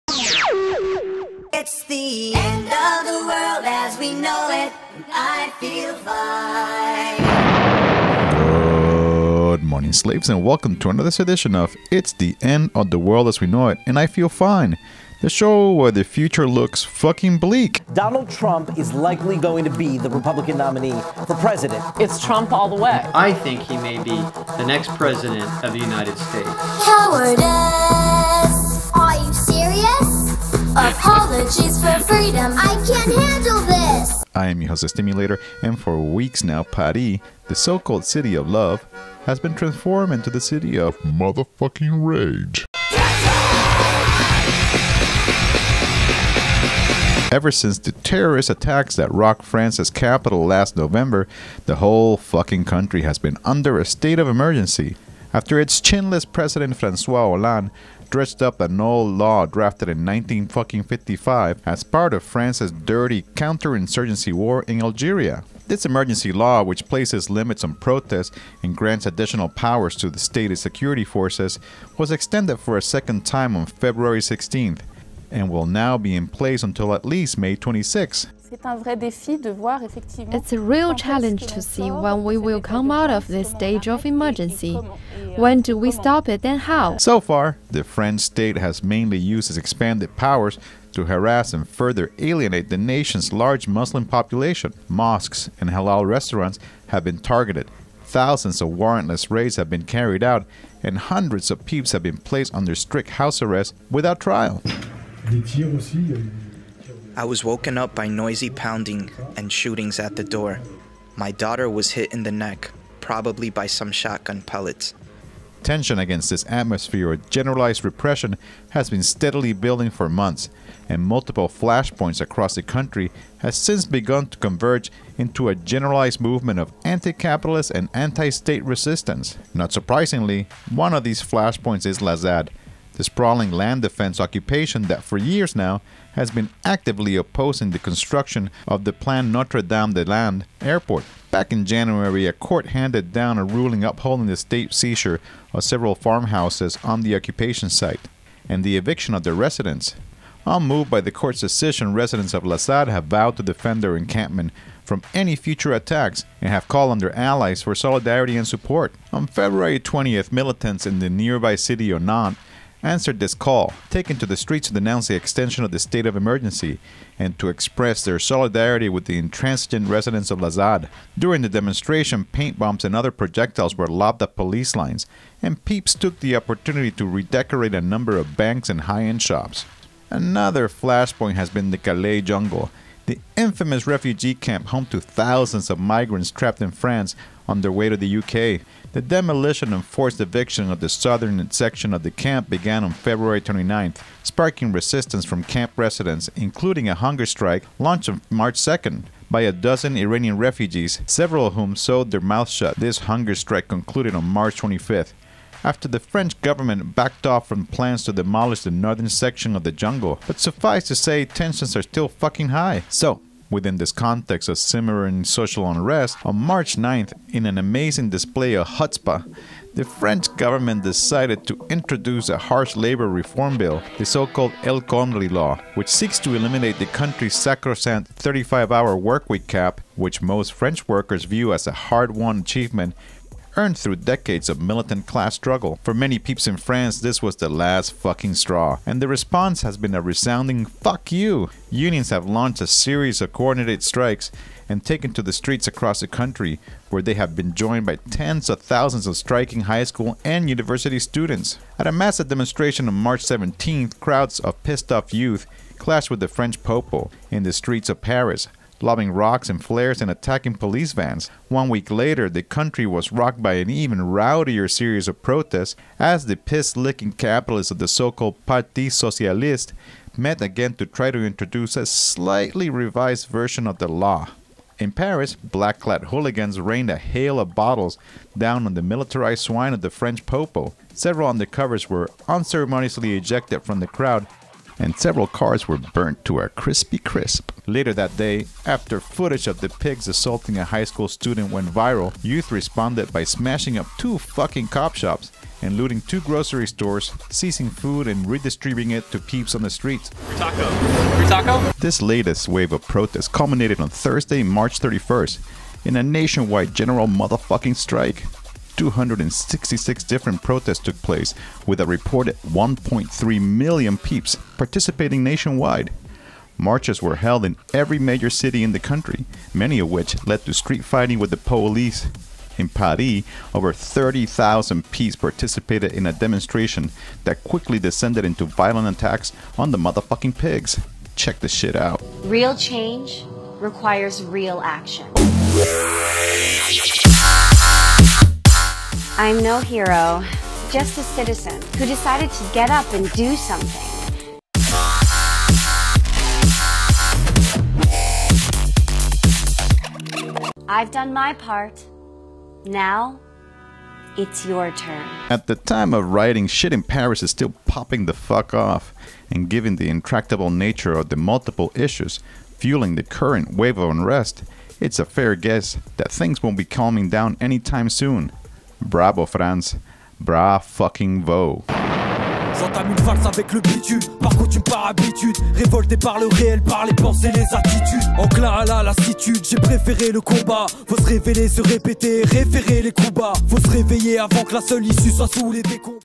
Paris is Burning
This week we take a look at the multiple converging flashpoints of resistance in France, which have combined into a popular movement that has inspired over a million peeps to take to the streets in a massive expression of collective rage.